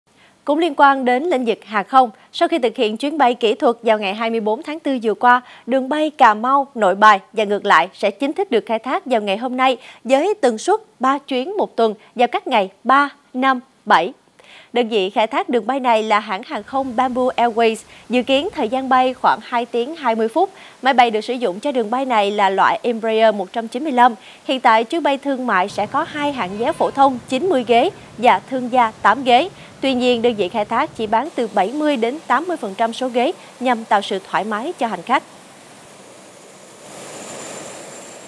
✈️✈️✈️ Hôm nay chính thức khai thác đường bay Cà Mau - Hà Nội
kéo_co ết_nối_miền_tây